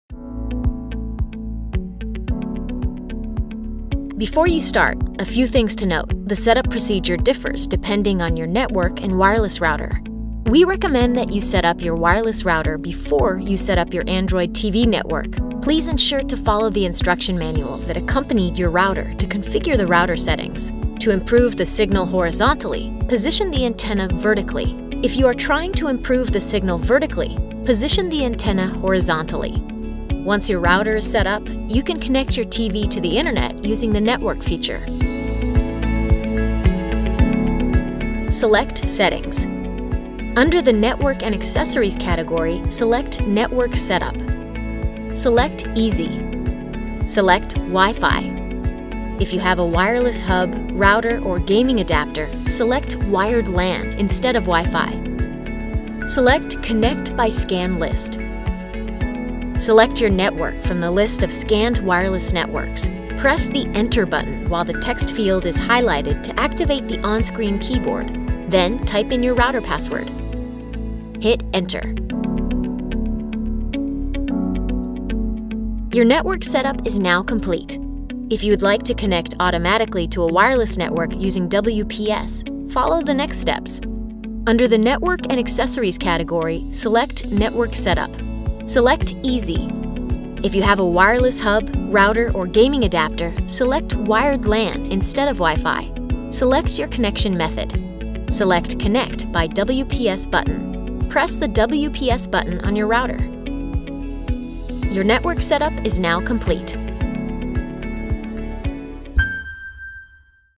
How To | Connect your Sony Android TV to a wireless network
Learn how to connect your Sony Android TV to a wireless network.